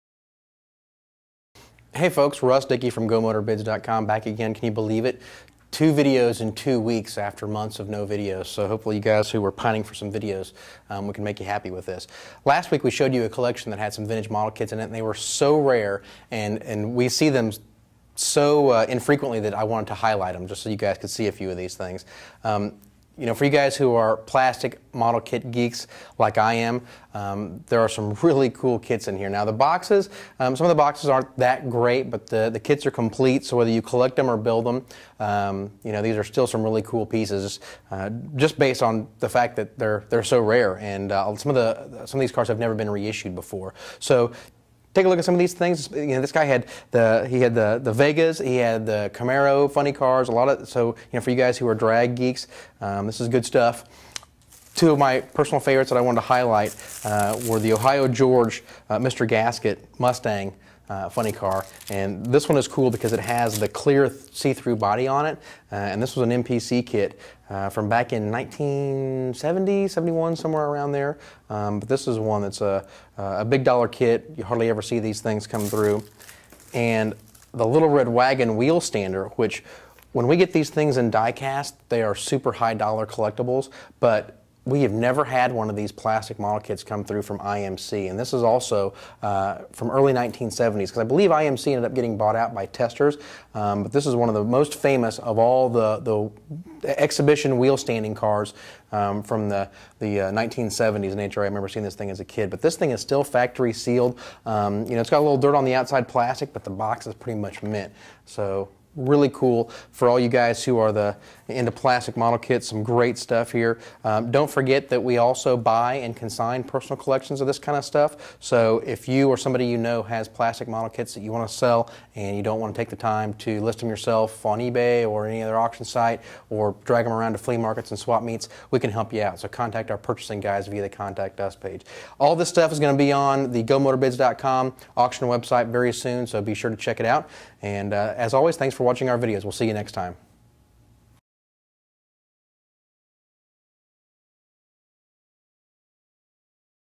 Vintage Drag Racing Plastic Model Kits From the 60s and 70s
Whether you collect them or build them, these rare drag racing model kits from the 1960s and 70s may be just what you are looking for. These pieces will be crossing the virtual auction block of the GoMotorBids website at the end of August, beginning of September. Don't forget, we are always looking to purchase or consign private collections of plastic models just like these!